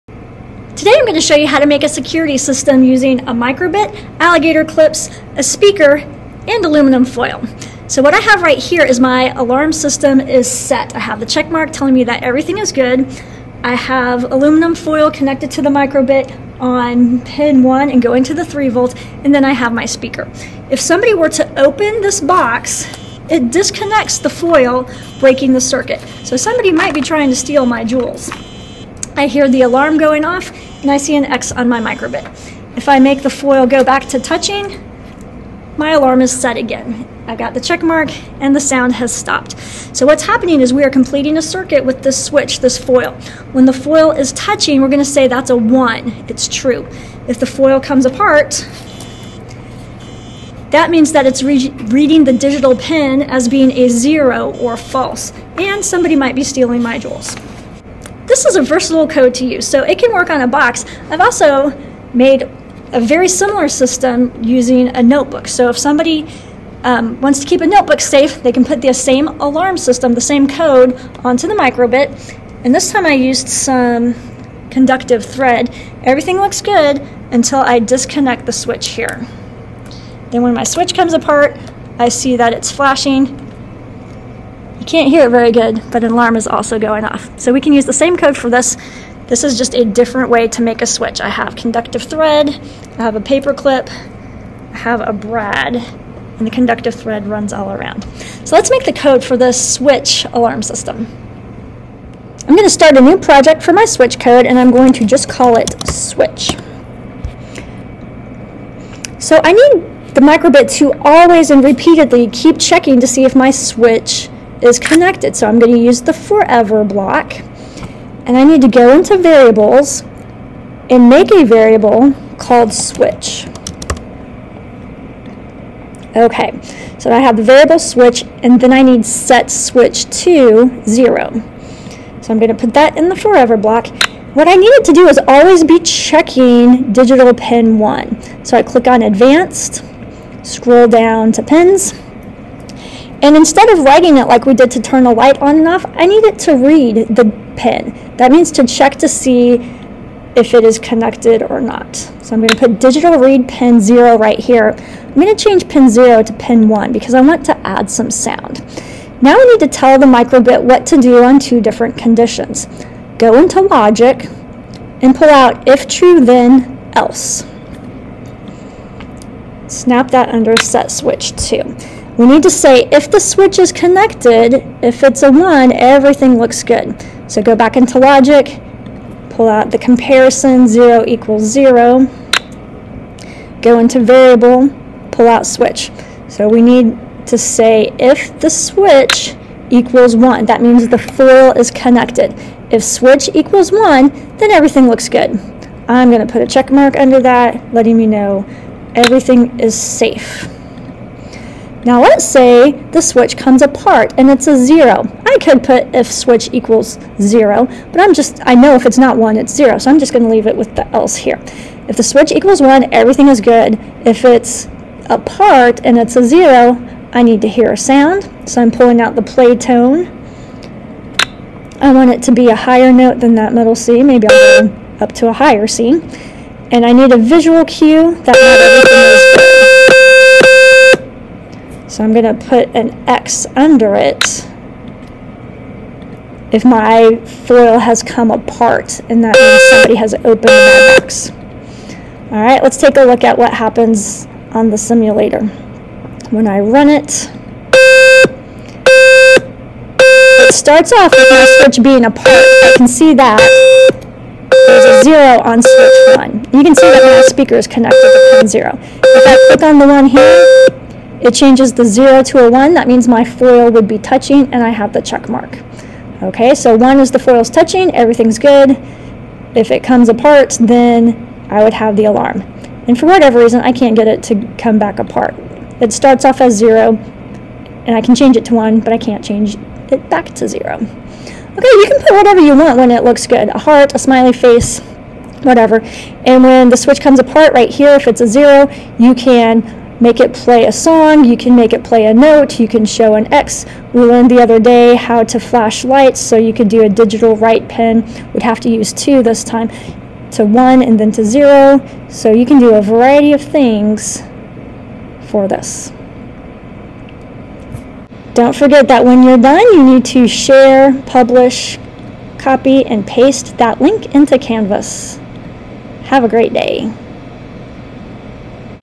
Switch Alarm and Code - Updated Version
Make an alarm system using a micro:bit, aluminum foil, speaker, and alligator clips!